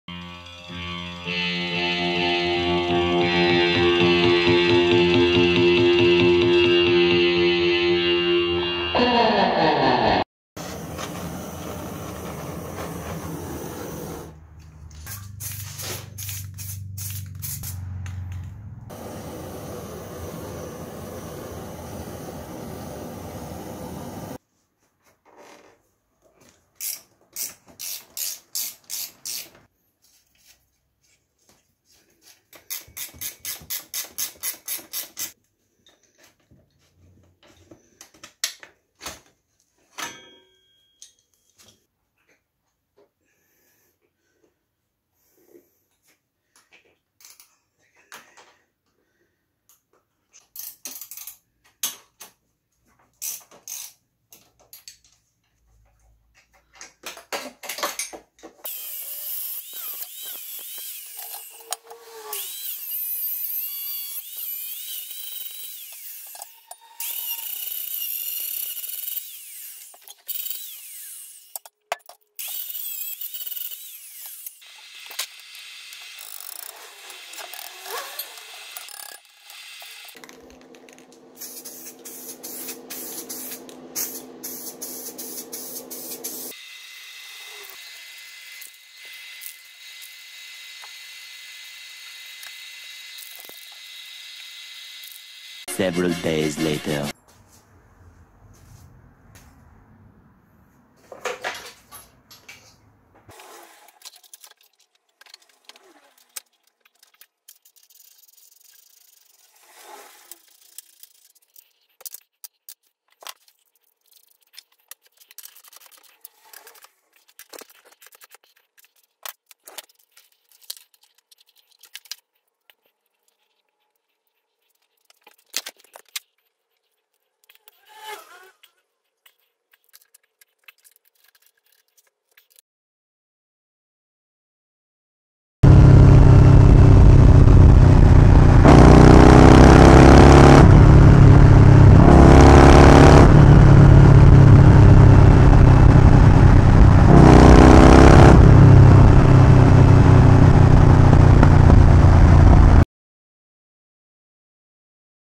엑시브 250 카페레이서 프로젝트19
뒷차대 다듬기 및 도색.

전체 재생목록